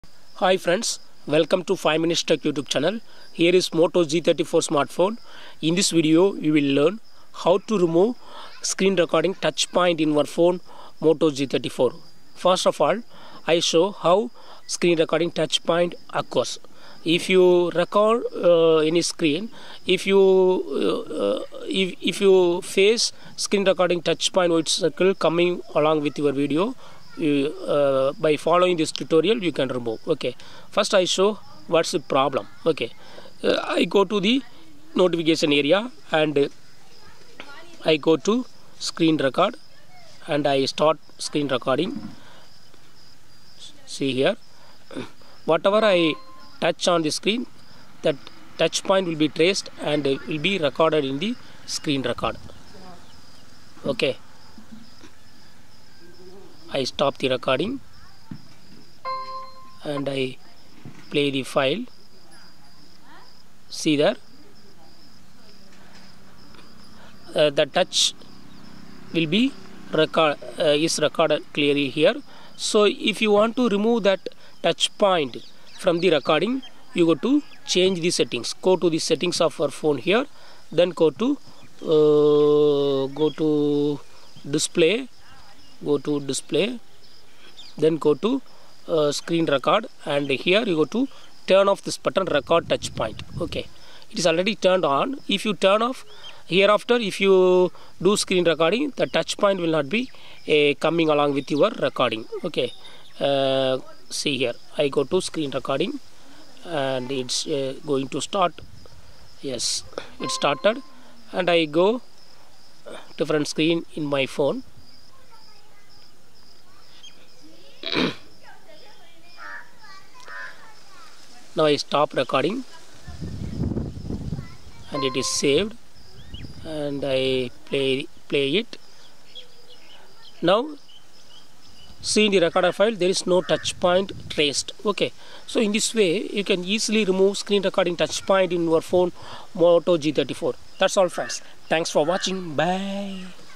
How to remove screen recording touch point in Moto G34 5G
Moto G34 5G screen recording touch point remove settings: Learn here how to remove screen recording touch point in Moto G34 5G smartphone.